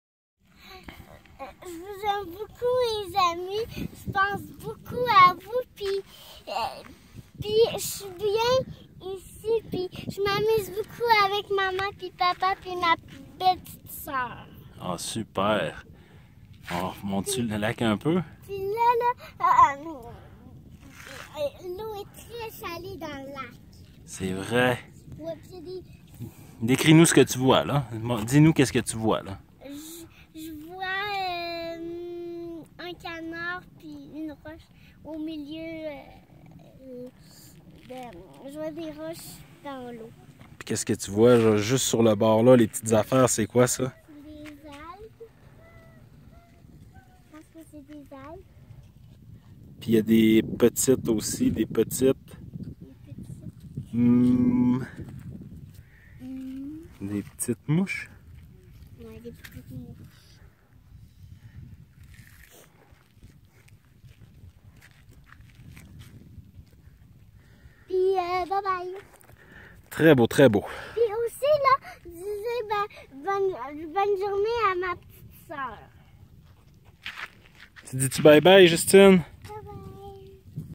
Gabrielle en Californie
Gabrielle s'adresse à ses camarades de maternelle et à son professeur lors de son voyage en Californie